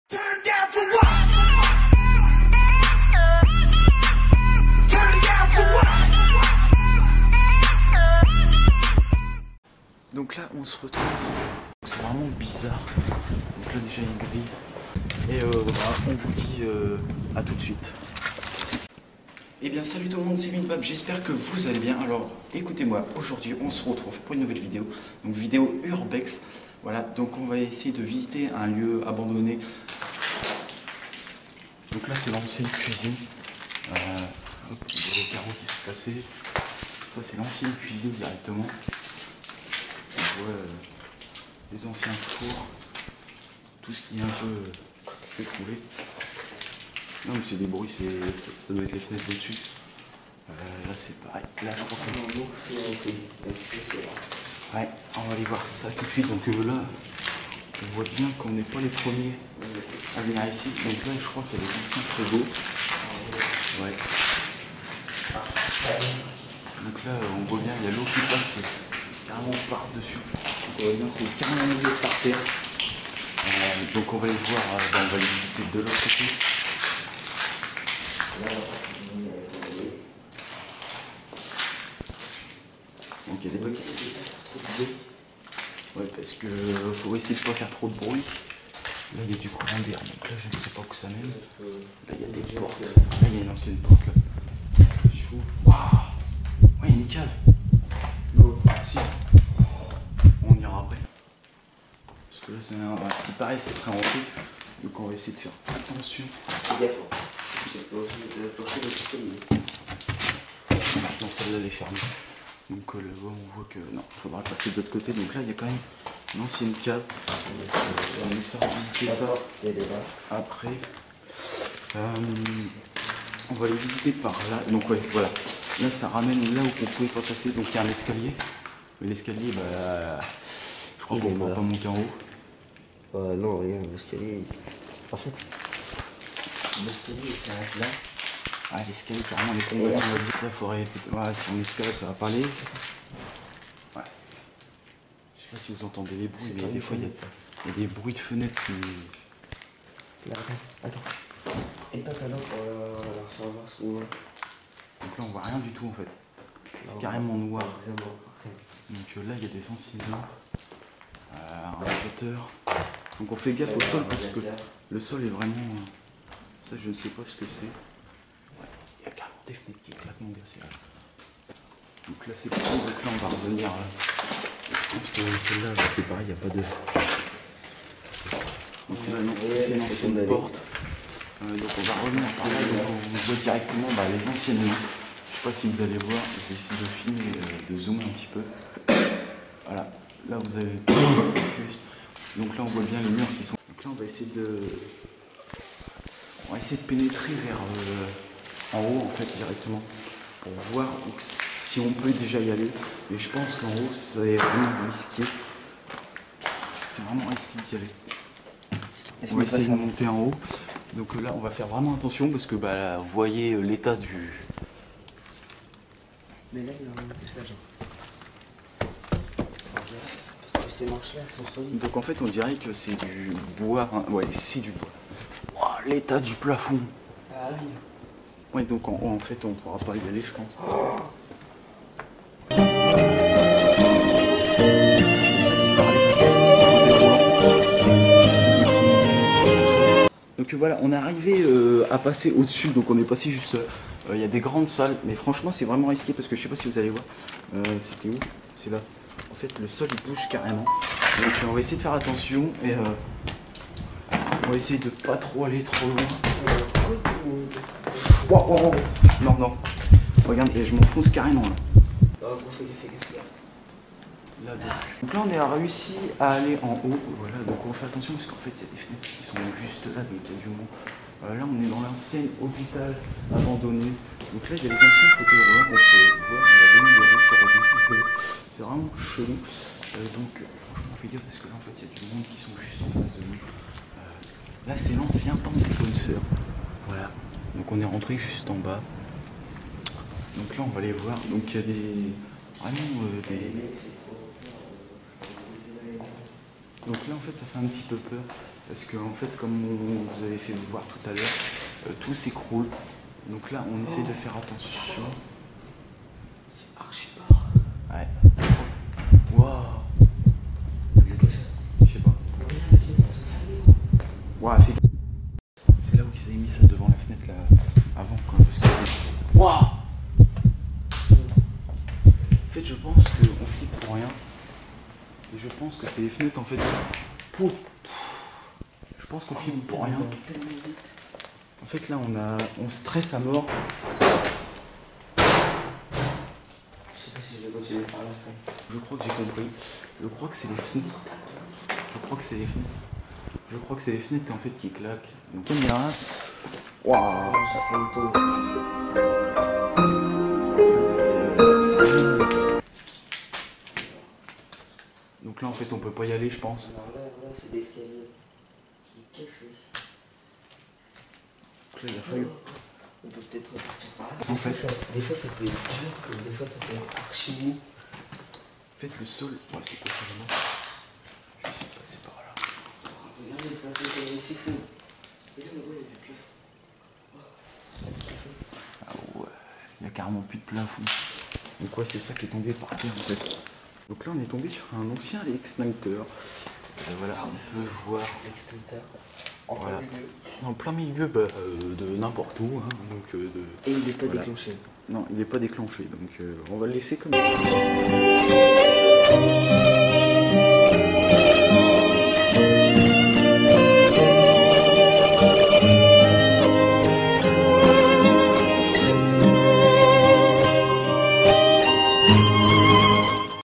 URBEX BRUIT BIZAR #1
on visite un lieux abandonné vraiment magnifique la suite de la vidéo arriveras prochainement abonné vous likez commenté partager pour de prochaine vidéo urbex